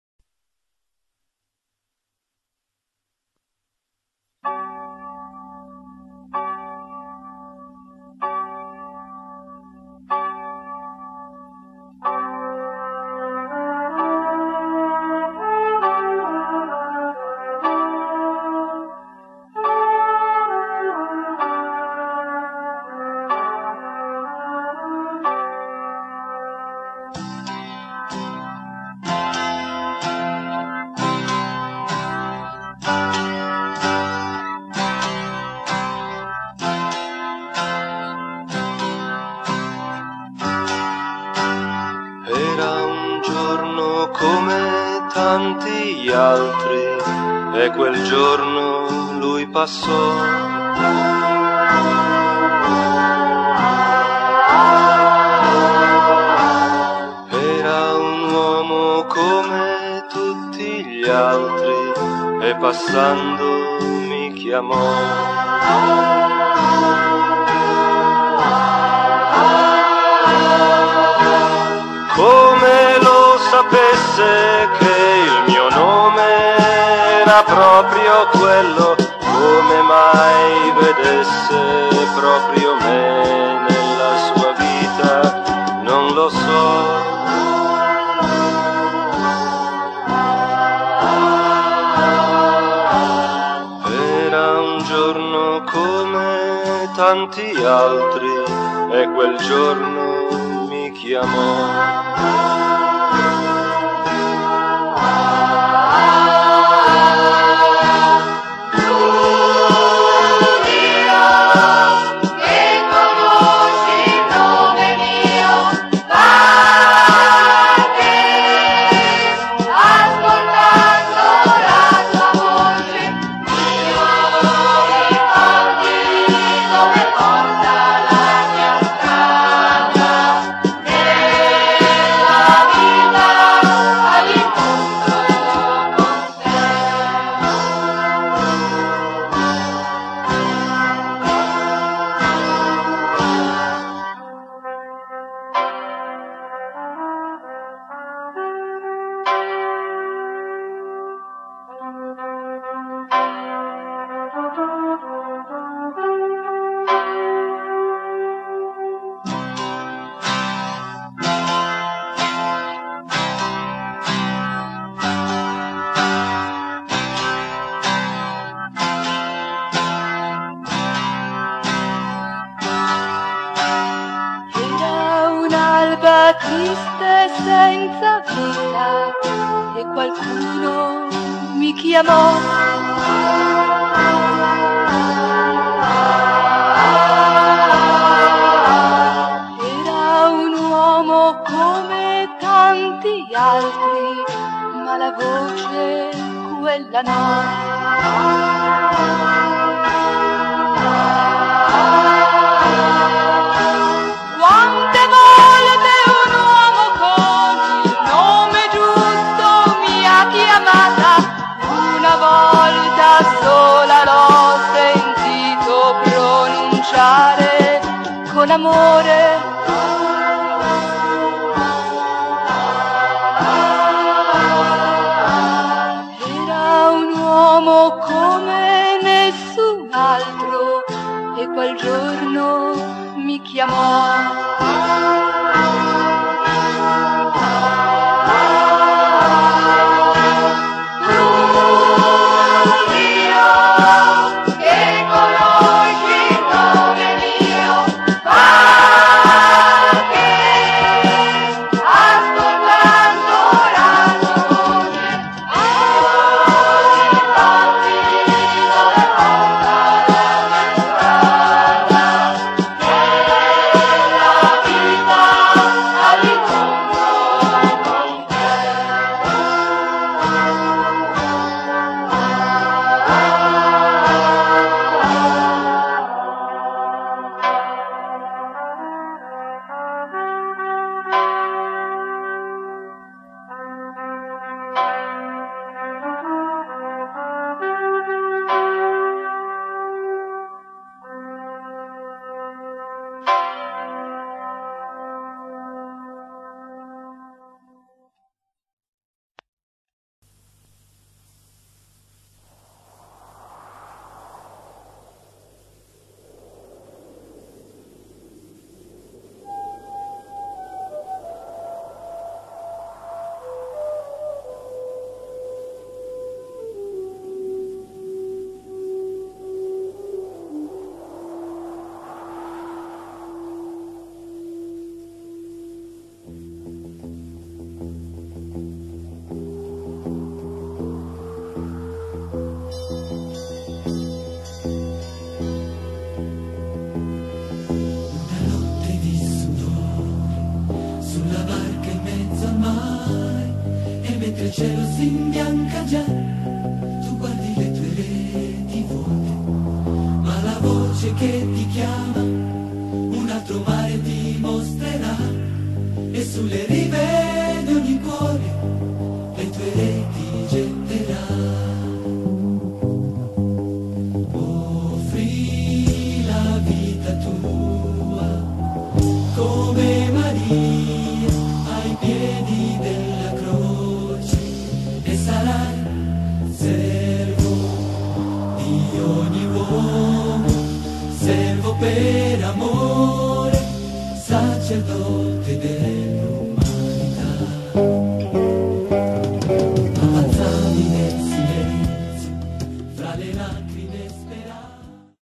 Photostory ordinazione diaconale Federico Battaglia
PhotoStory della nomina a diacono di Federico Battaglia - 26 settembre 2010. Duomo di Napoli